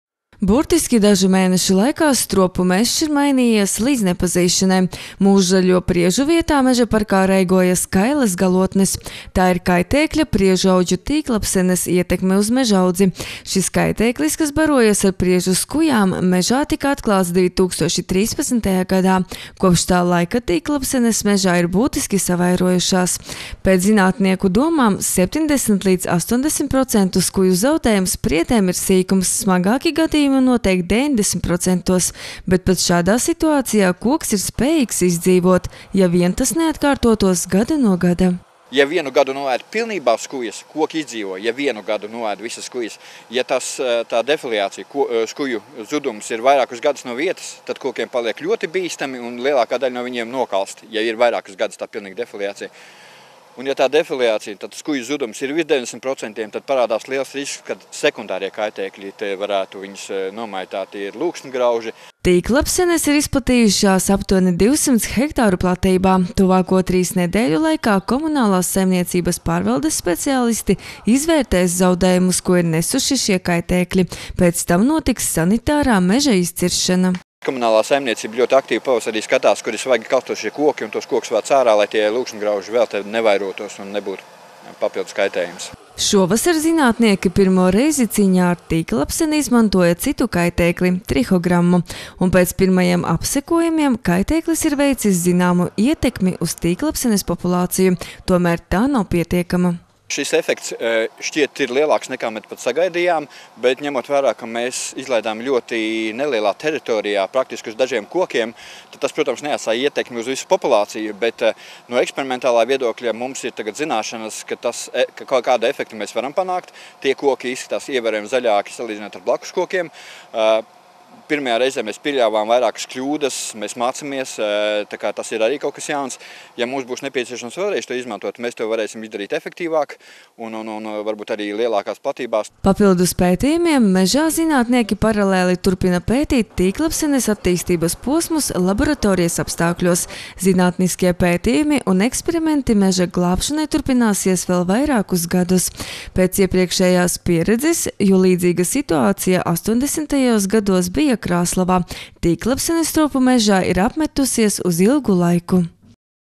Tīkllapsenes uzbrukums Stropu mežam turpinās
Stropu mežā aptuveni 200 hektāru platībā ir izplatījušās tīkllapsenes. Komunālās saimniecības pārvaldes speciālisti izvērtēs zaudējumus un veiks sanitārā meža izciršanu.